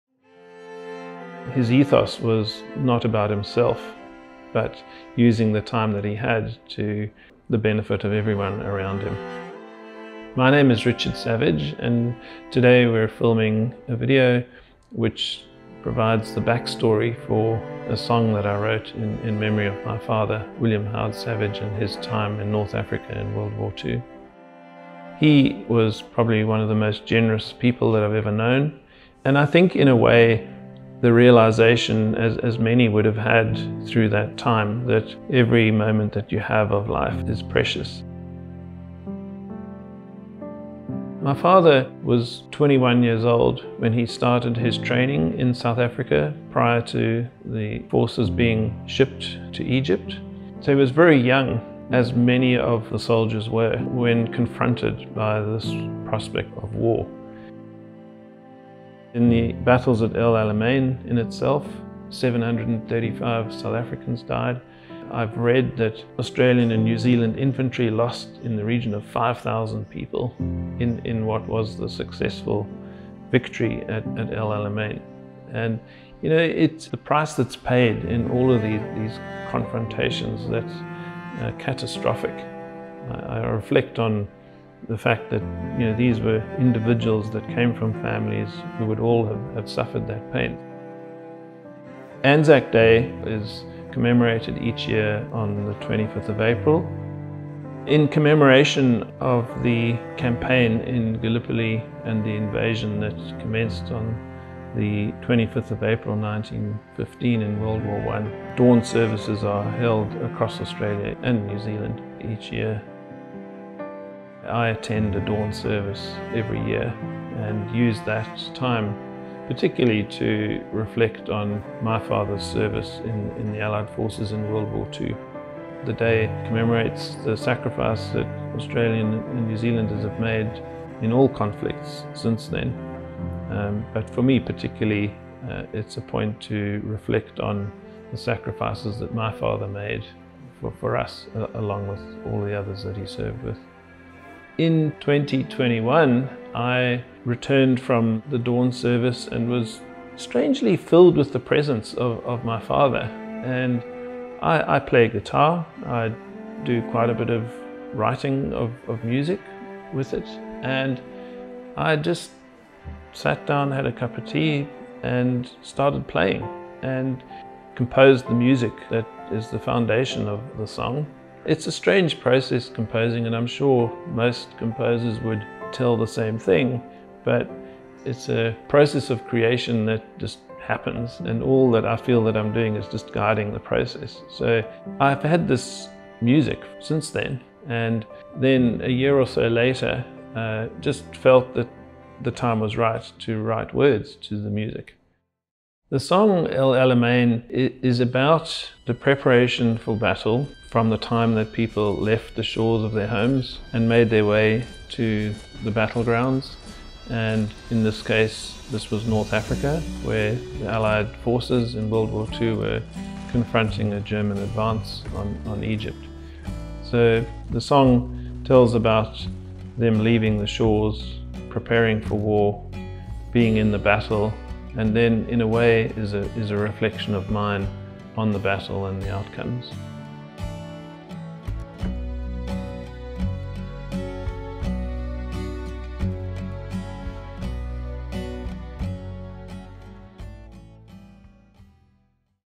El Alamein Mini Documentary Richard H Savage
This video tells the back story to the song about my father's service in the Allied Forces in the North Africa Campaign in World War II and specifically, the Battles of El Alamein, Oct-Nov 1942.
Videography and production by  TEMENOS Production House, Gold Coast, Australia